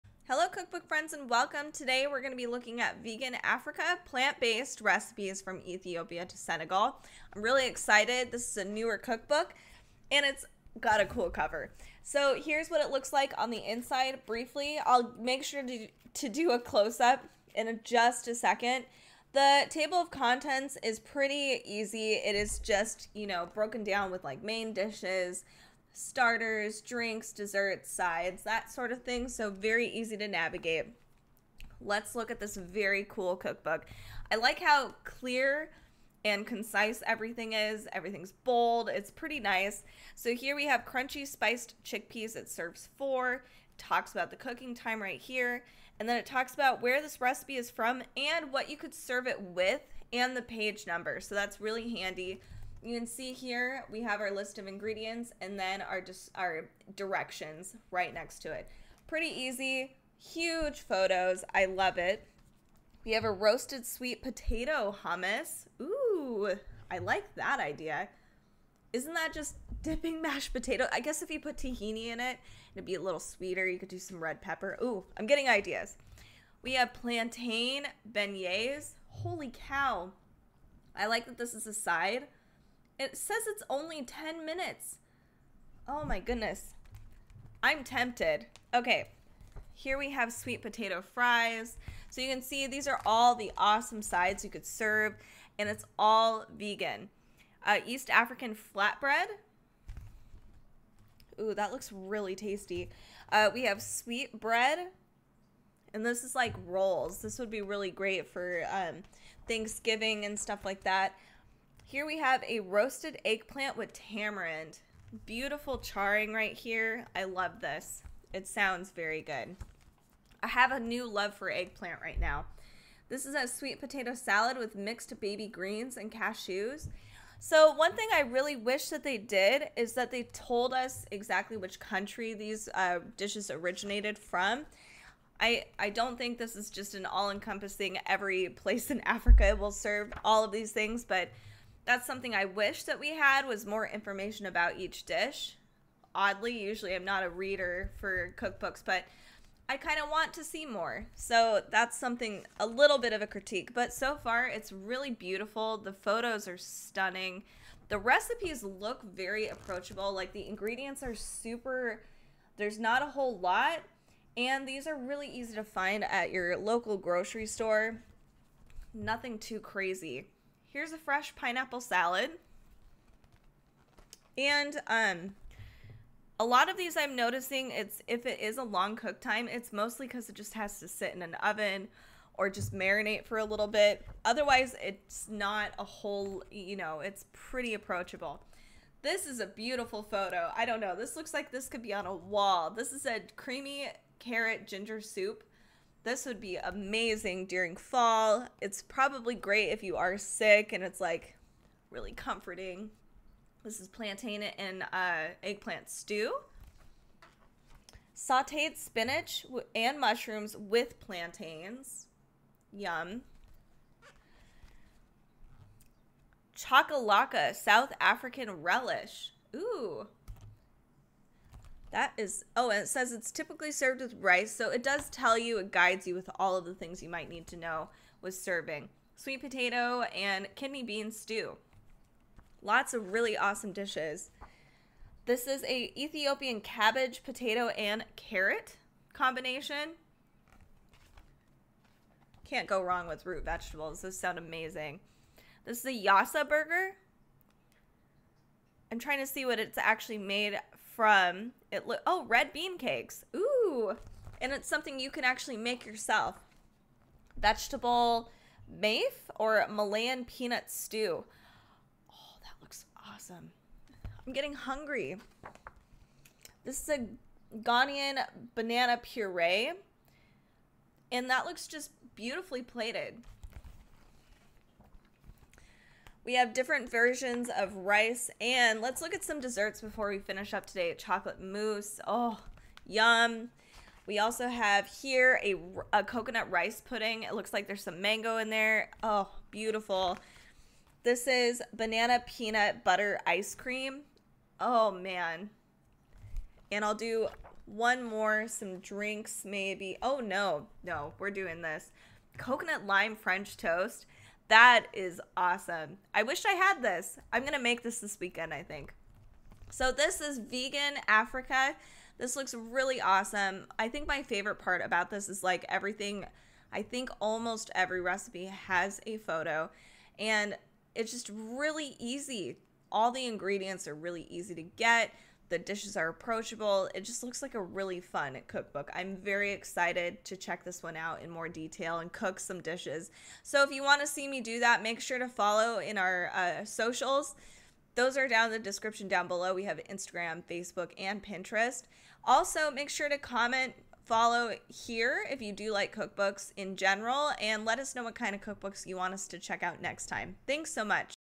Cookbook Preview: Vegan Africa: Plant-Based Recipes from Ethiopia to Senegal, by Marie Kacouchia
Here is my cookbook preview of "Vegan Africa: Plant-Based Recipes from Ethiopia to Senegal", by Marie Kacouchia (The Experiment, 2022)